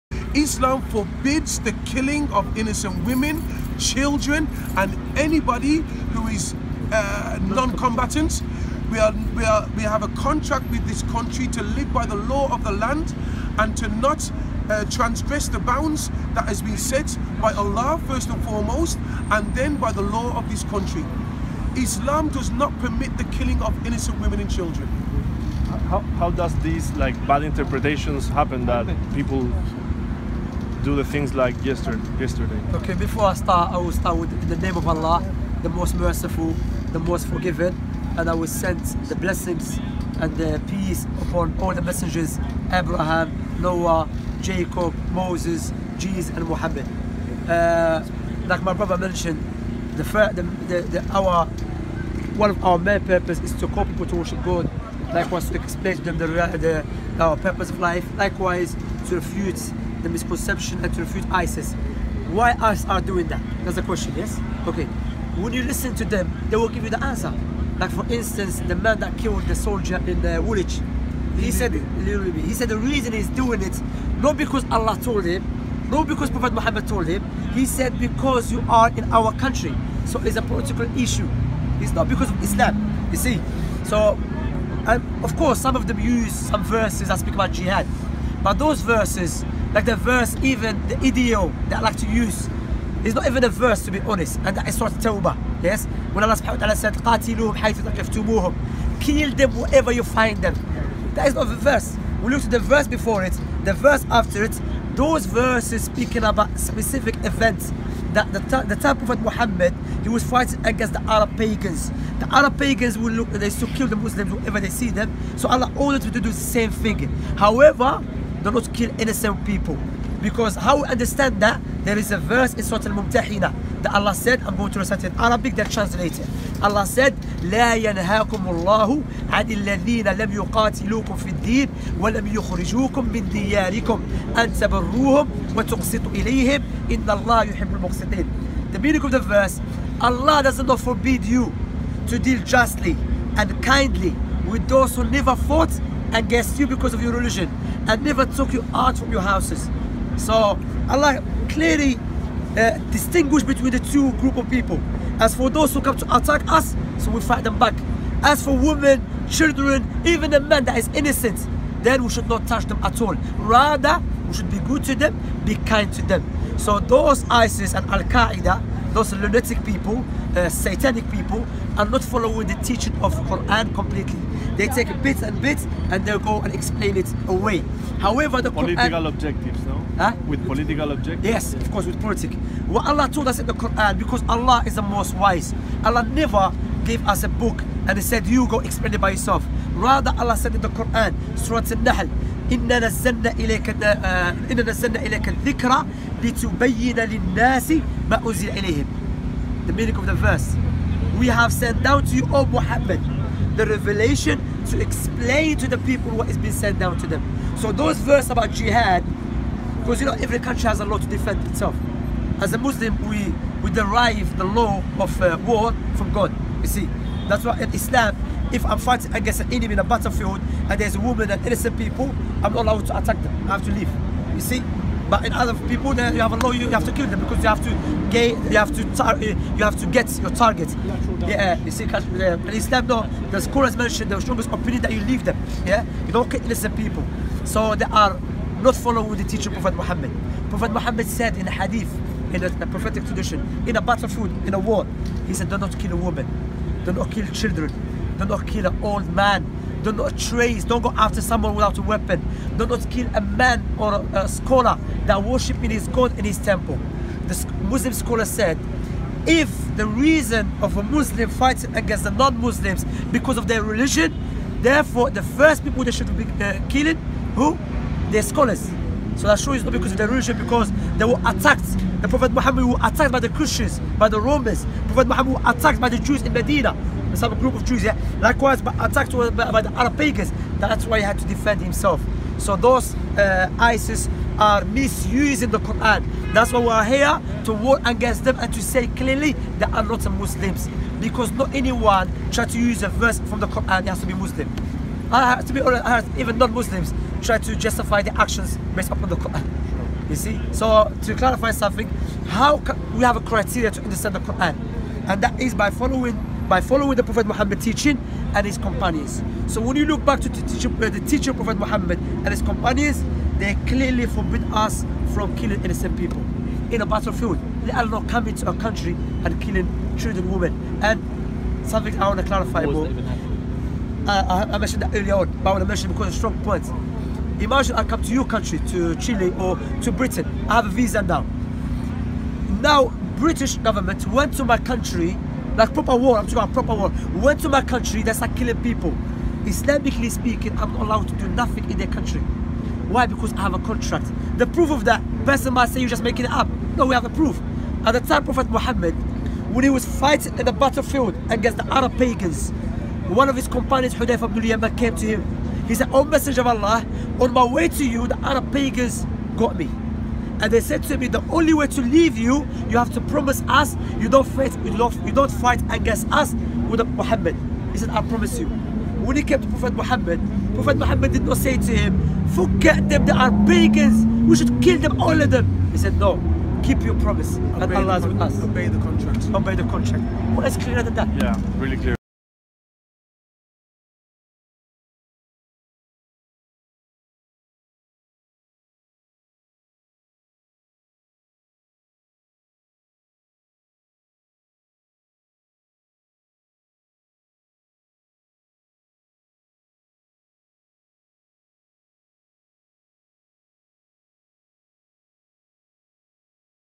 (Explicación clara) Por qué el Islam no es terrorismo
Esto corresponde en Londres, en donde musulmanes han montado puestos y mesas de información en la calle para explicar a la gente que el terrorismo no tiene nada que ver con el Islam... y condenamos por ello enérgicamente el atentado en Barcelona.

Comparte para parar la islamofobia, y el rechazo del Islam y de los musulmanes, los cuales somos también las principales víctimas de estos sucesos... atentados, perpetrados en Barcelona...

Paz :)